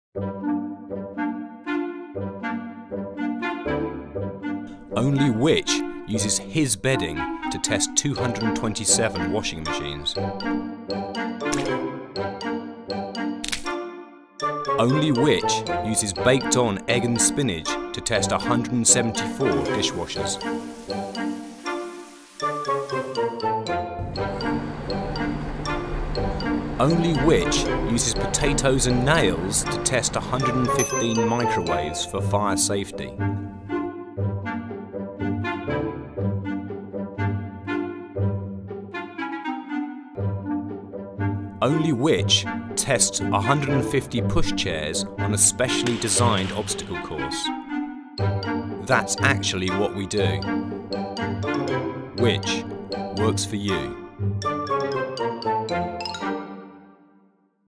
Which? works for you - Washing Machine, Dishwasher, Microwave and Pushchair reviews
Only Which? tests washing machines with dirty dogs, only Which? tests dishwashers with baked on food, only Which? tests Microwaves with potatoes with nails in and only Which? tests pushchairs with a specially designed obstacle course.